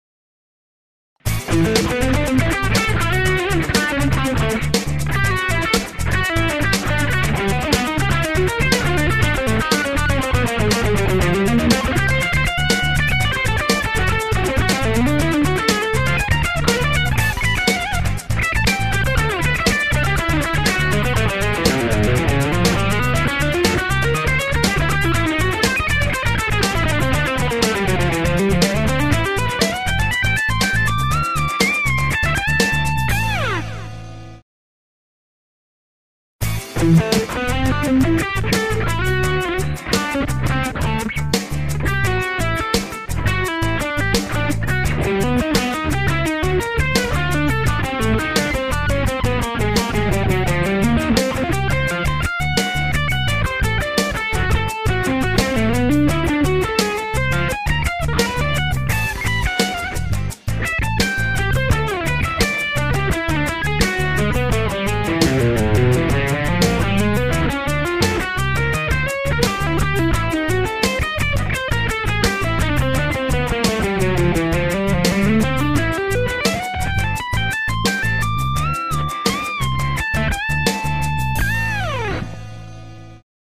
Greg Howe style outside phrasing Lesson by Andrea Accorsi
I hope this video can give you some idea about the outside  phrasing of G.H.